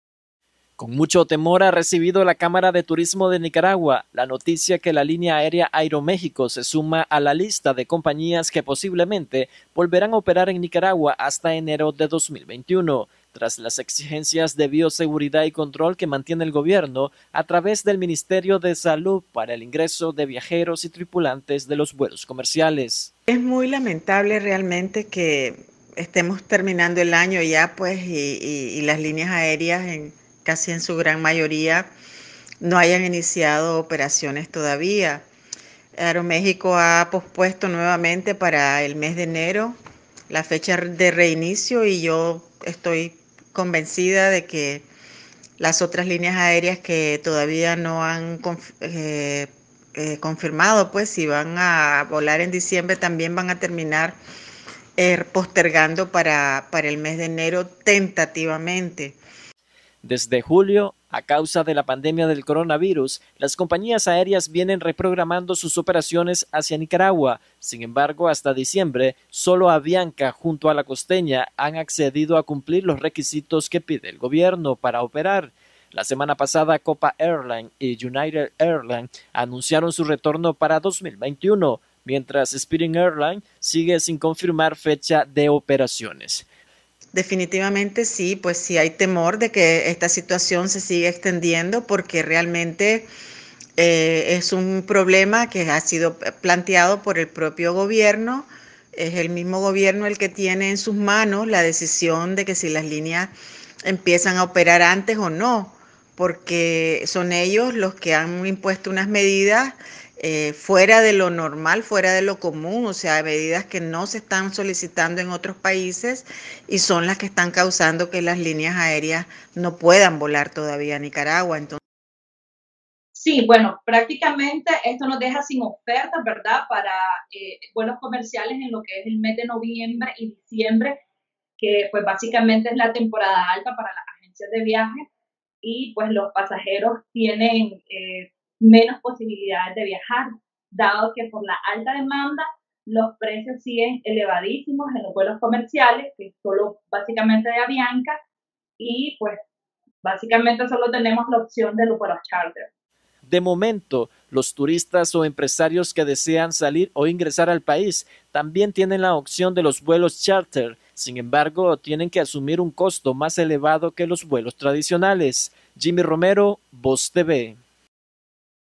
NOTIVOS || Aeromexico decide no regresar a Nicaragua este 2020
Aeroméxico se suma a la lista de aerolíneas que posponen su retorno a Nicaragua, con una fecha tentativa de regreso hasta el 14 de enero de 2021.